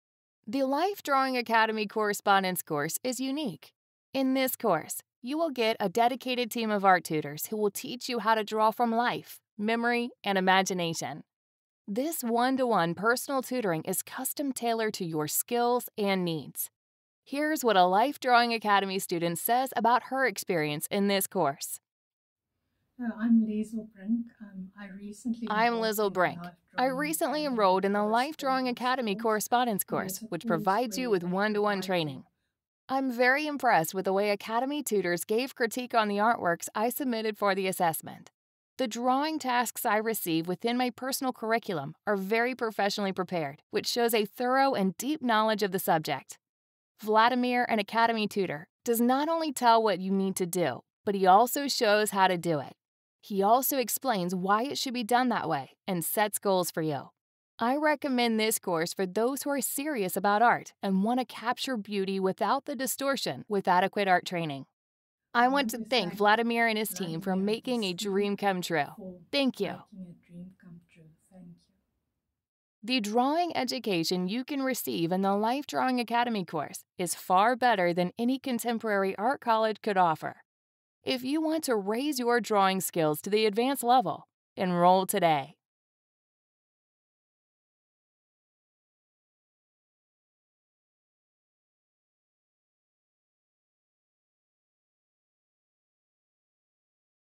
Life Drawing Academy Correspondence Course review
- The Life Drawing Academy Correspondence Course is unique. In this course, you will get a dedicated team of art tutors who will teach you how to draw from life, memory and imagination. This one-to-one personal tutoring is custom tailored to your skills and needs.

Here's what a Life Drawing Academy student says about her experience in this course: "I’m Liezl. I recently enrolled in the Life Drawing Academy Correspondence course, which provides you with one-to-one training. I’m very impressed with the way academy tutors gave critique on the artworks, I submitted for the assessment. The drawing tasks I receive within my personal curriculum are very professionally prepared, which shows a thorough and deep knowledge of the subject. Vladimir, an academy tutor, does not only tell what you need to do, but he also shows how to do it. He also explains why it should be done that way and sets goals for you. I recommend this course for those who are serious about art, and want to capture beauty without the distortion with adequate art training. I want to thank Vladimir and his team for making a dream come true. Thank you!"

The drawing education you can receive in the Life Drawing Academy course is far better than any contemporary art college could offer.